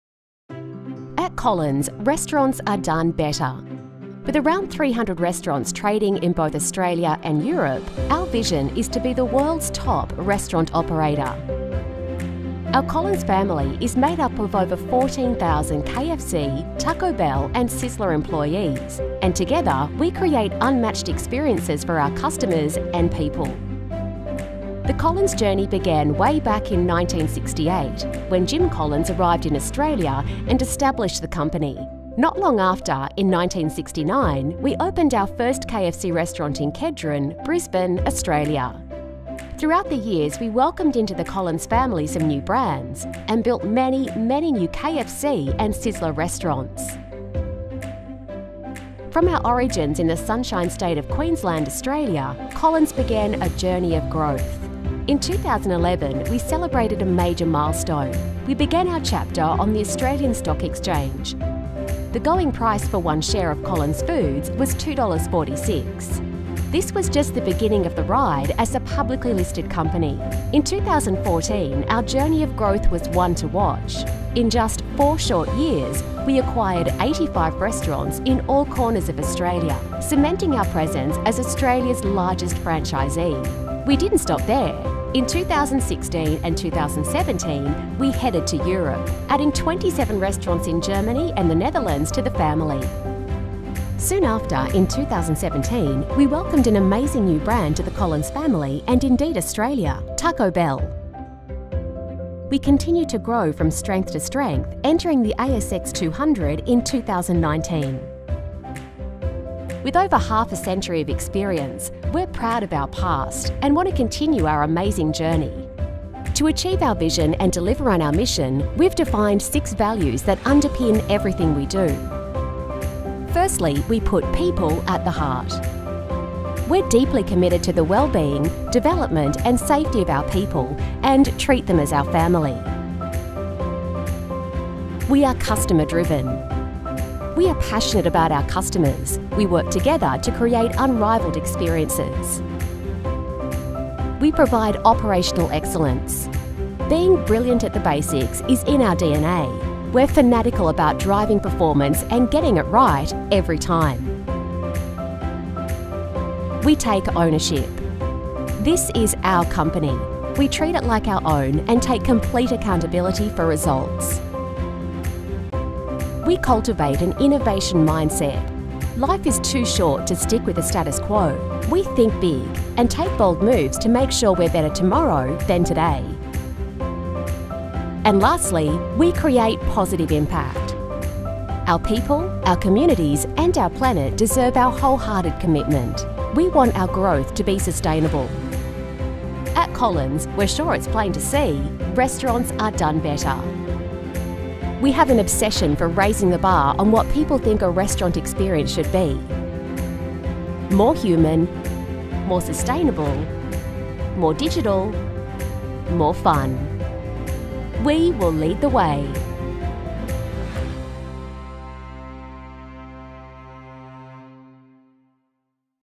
The Collins Way Story
At Collins, we are proud of our rich history and have bold ambitions as we look to the future. Join us as we take you on our journey growth and introduce you to our organisational values. We're committed to raising the bar on what people think a restaurant experience should be - more human, more digital, more sustainable, more fun. We will lead the way!